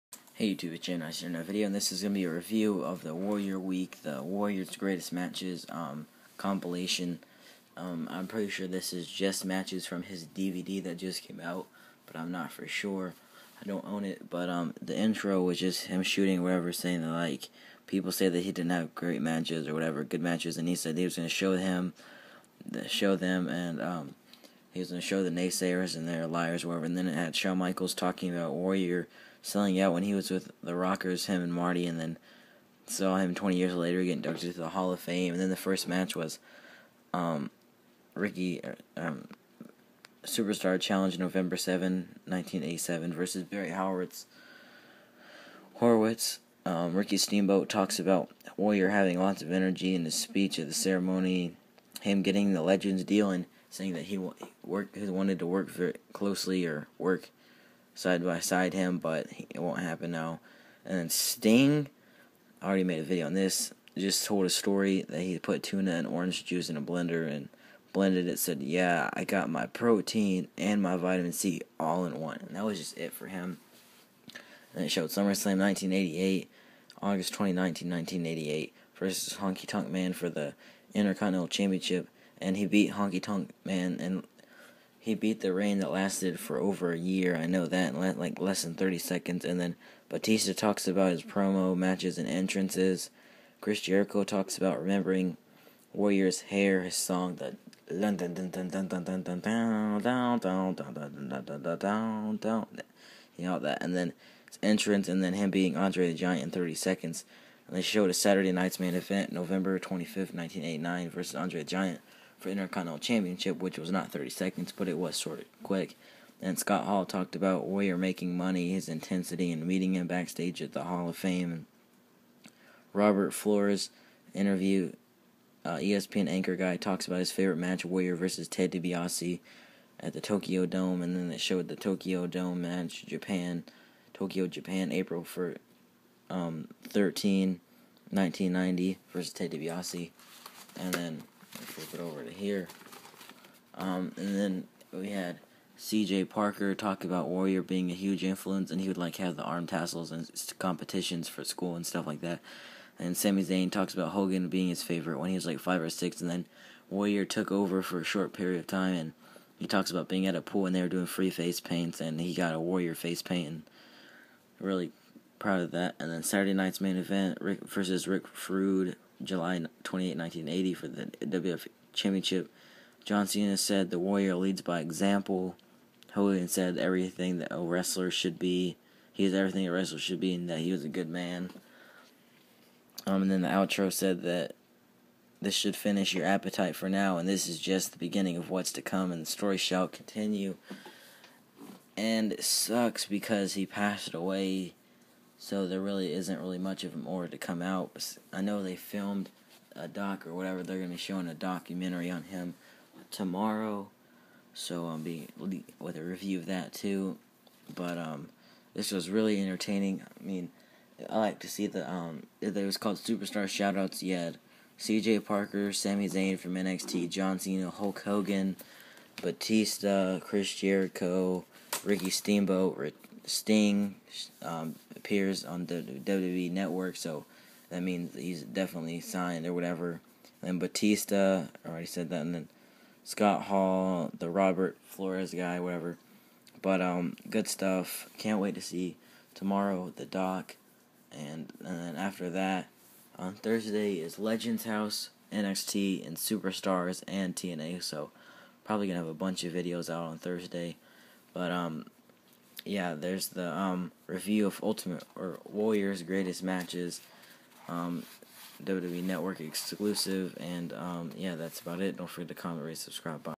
WWE Ultimate Warrior's Greatest Matches : Review [WWE Network Special]
Really entertaining.